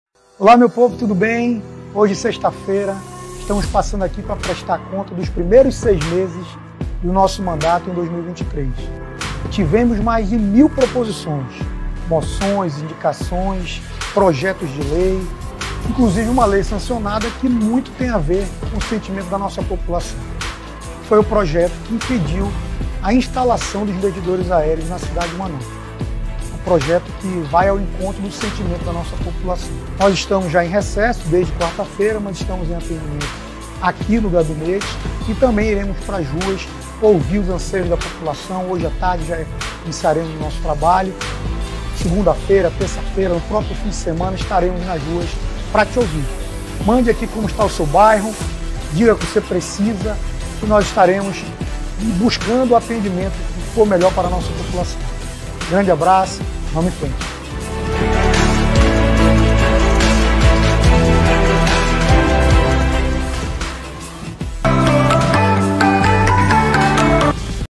Portal IF3M I Presidente da CMM Caio André presta contas dos últimos seis meses de mandato
🔴Portal IF3M - Presidente da CMM Caio André presta contas dos últimos seis meses de mandato

🔊SAIBA MAIS🔊

 • Portal IF3M I Pod...

🪀 Entre no nosso grupo de WhatsApp
🔊AGORA🔊